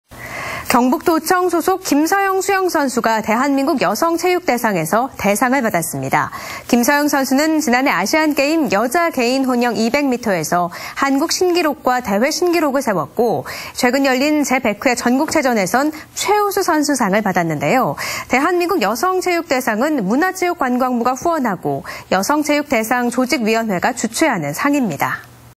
경북도청 김서영 수영선수, '여성체육대상' 수상 / 안동MBC
2019/11/26 10:05:09    작성자 : 최보규

경북도청 소속 김서영 수영선수가
오늘 열린 대한민국 여성체육대상에서
대상을 받았습니다.
◀ＥＮＤ▶

김서영 선수는 지난해 아시안게임에서
여자 개인혼영 200m 부문 한국 신기록과
대회 신기록을 세웠고,
최근 열린 제100회 전국체전에서는
최우수선수상을 받았습니다.

대한민국 여성체육대상은
문화체육관광부가 후원하고
여성체육대상 조직위원회가 주최하는 상으로, 한 해를 빛낸 여성 체육인을 선정해 격려하고 있습니다.